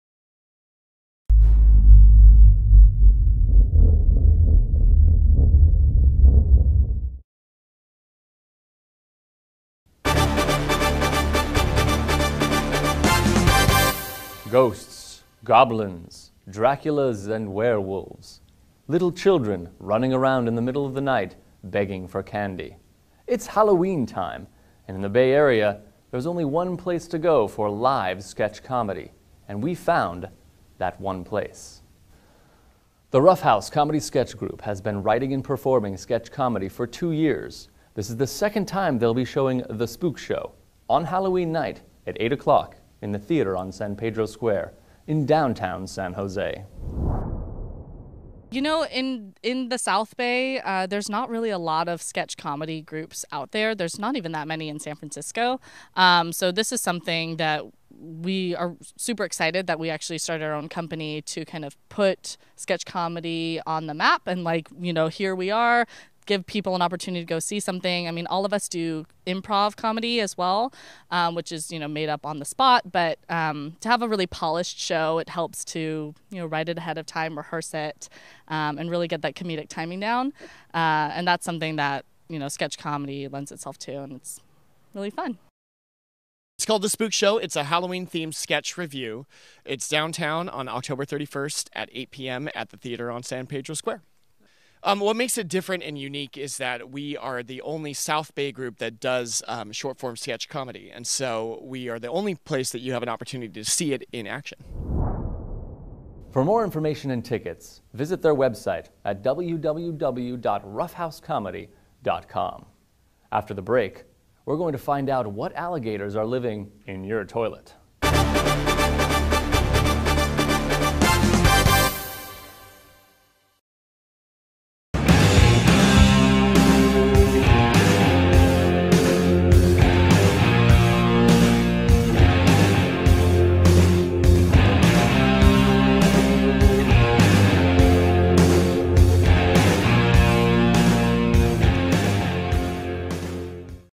The Spook Show News Coverage - Presented By The Production League
News coverage of Rough House Comedy's Spook Show in San Jose

Instagram Name: theproductionleague